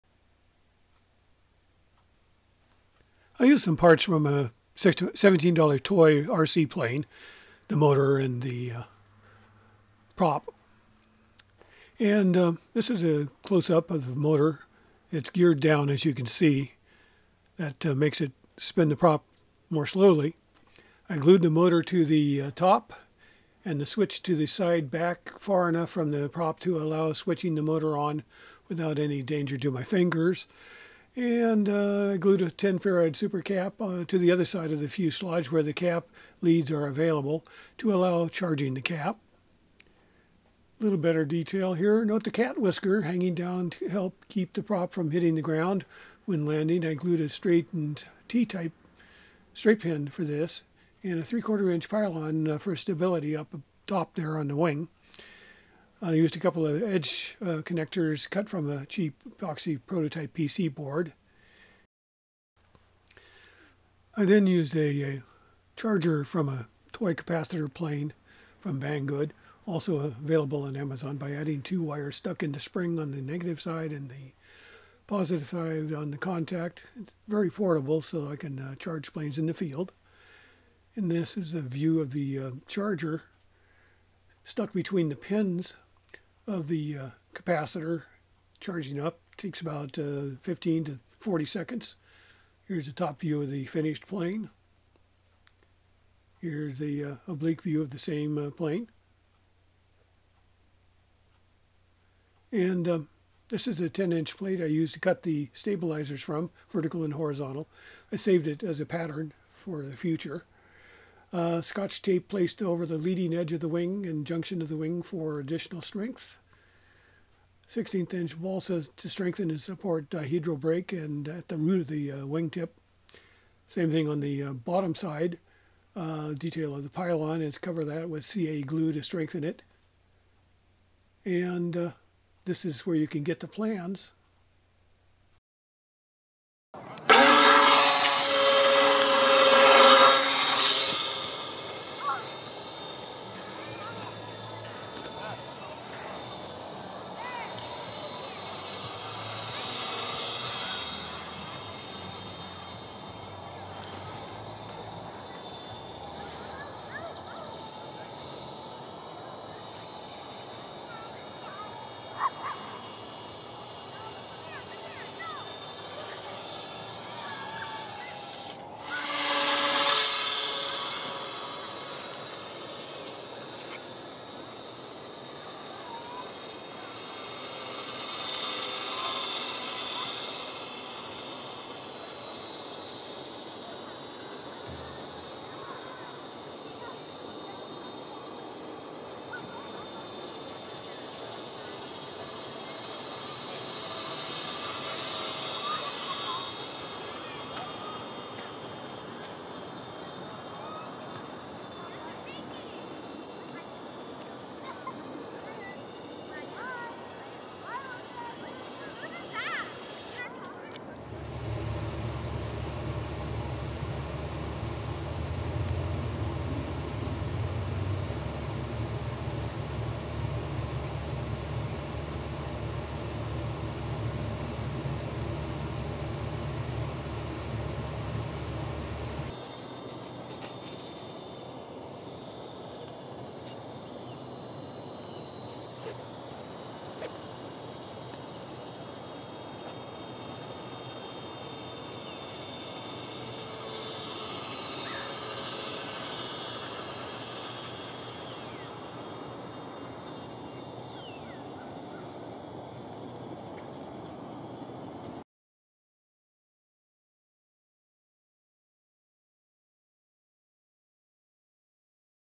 Aero Drifter Build, Flying as a Capacitor Plane
This video shows how I modified Bob DeShields Aero-Drifter Free Flight HLG to use a small motor powered by a Super Capacitor.

UPDATE:  The motor I used is one similar to one available from Banggood that uses (7 x 20mm) motor, a 135mm prop on a gear reduction unit.  If your model needs more power, there are 8.5 x 20mm motors available that might work well with or without gearing them down to a slower propeller speed.  Slow speed geared motors work better with larger props.  7 x 20mm (720) using only the typical 55-65mm prop at full speed may or may not work.  The geared down versions (720 or 8520) with 135mm props work. Some have used two, counter rotating 720 or 8520 motors  spaced a few inches left and right of the center line mounted on the wings using the usual 55 or 65mm props designed for use with these motors.

The plane uses a Spruce fuselage with Foam Plate Vertical and Horizontal Stabilizers and a Sand Paper Shaped 3mm Foam board for the main wing (paper removed).  The stabilizers and main wing have trim tabs to allow setting a limit on the free flight circle to match the flying space where I fly this model.  Keep in mind that a tighter circle will tend to make the plane come down sooner or even dive if not compensated by up elevator adjustment.

I also used clear packing tape on the leading edge of the main wing as well as at the dihedral breaks and wing center to strengthen these areas.

By the way, while the Aero-Drifter crashed into the tall pine tree at the end of this video, fortunately it fell through the branches and landed on the grass.  An earlier version was "treed" about two months ago but when I was finally able to knock it free from the 40 foot level using a long, telescoping pole, it actually flew free of the tree and landed nicely on the grass.  There was no serious damage.

 I should know better than to fly too high at this site if there is any wind at all.

I once modified one of the $8 kids capacitor planes (Banggood) with a 1/16th inch square balsa piece glued along the EPP fuselage to stiffen it up and using sandpaper to shape the wings into more of an airfoil shape only to have it fly up and over these same trees.  I watched it make nice circles as it rose higher and higher in a 5-7 MPH wind until it was just a microdot about 1/2-mile away... still circling nicely.  Bye bye!

Plans for DeShields original Balsa Wood Hand Launched Glider are